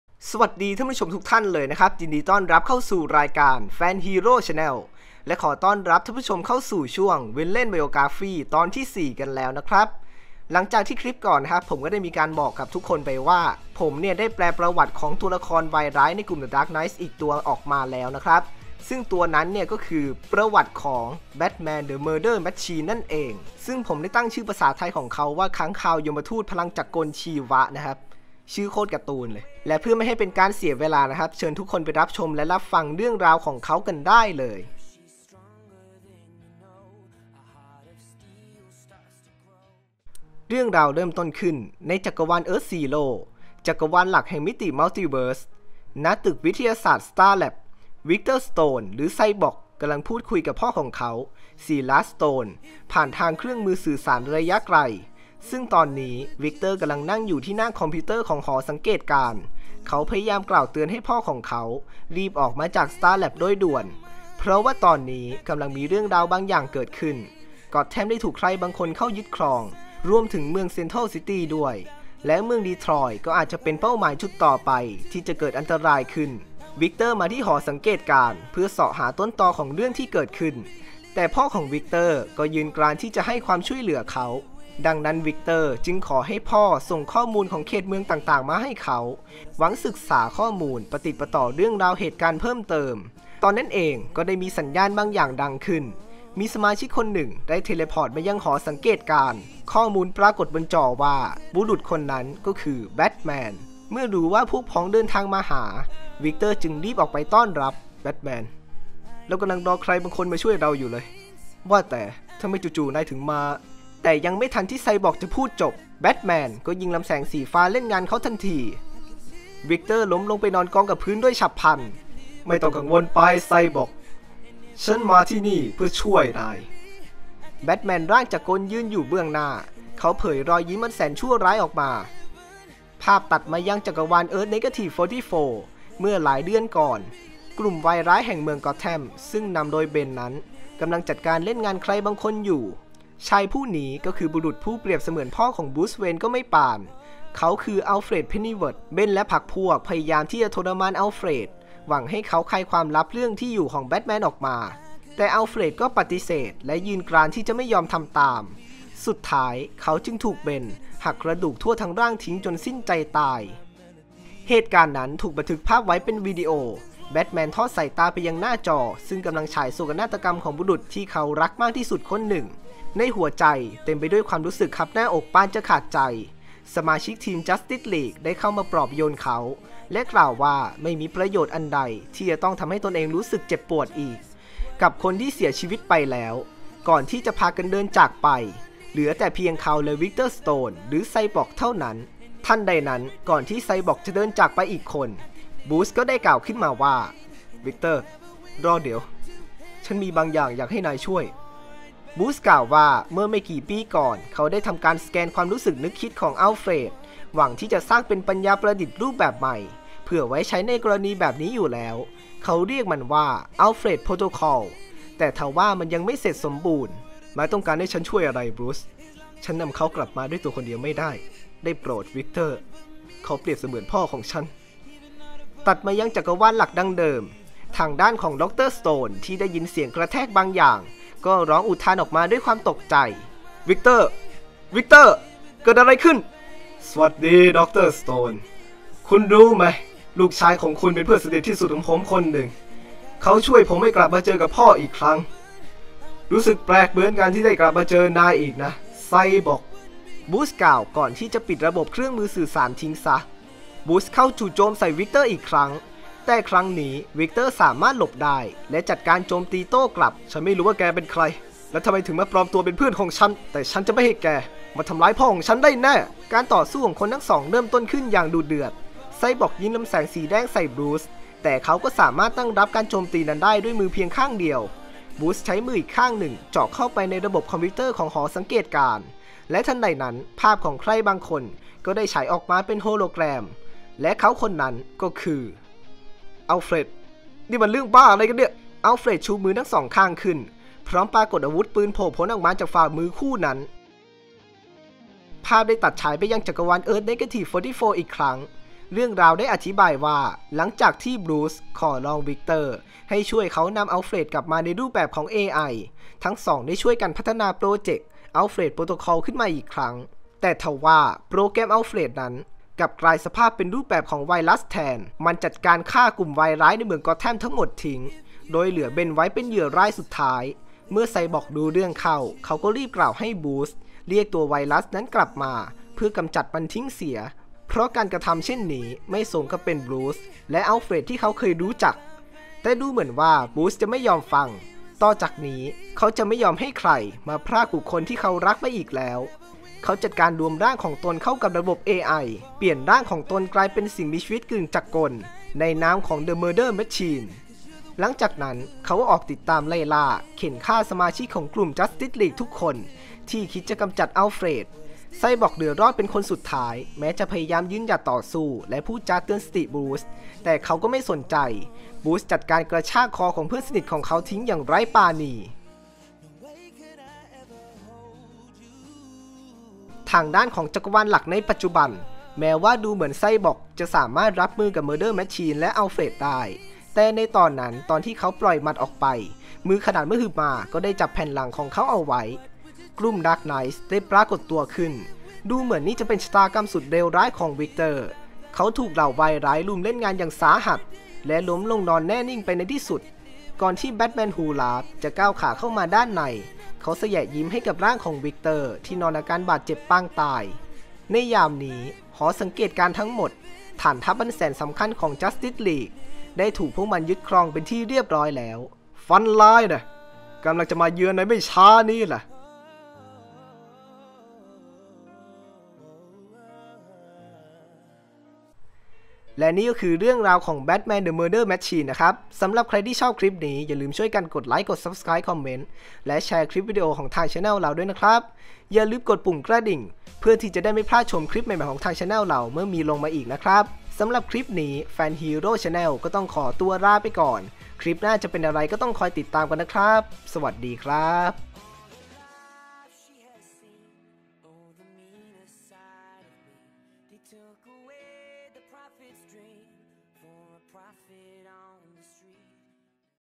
ประวัติ Batman: The Murder Machine ค้างคาวยมทูตพลังจักรกลชีวะ [FANHERO CH]
เครื่องจักรที่มนุษย์เราเคยใช้ควบคุมสั่งงาน บัดนี้ชะตากรรมของจักรวาลกลับถูกบงการโดยพลังเเห่งจักรกลชีวะ
 มหันตภัยอาวุธสังหารสุดร้ายกาจเเห่งสหัสวรรษ ได้พลันอุบัติขึ้นมาเเล้ว!!!